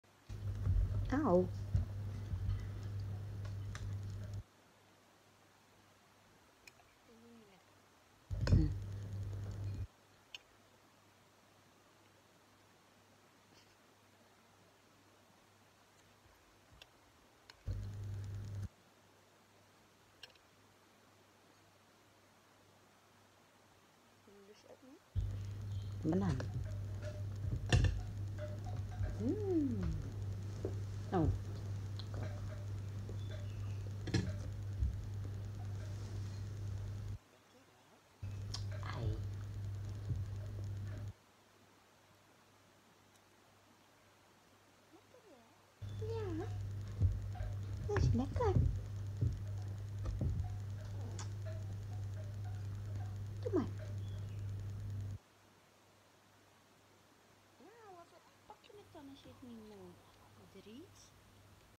Haantje en ik eten samen fruit.
Goeie morgen. De liefde van een haantje gaat door de maag.  Samen fruit eten.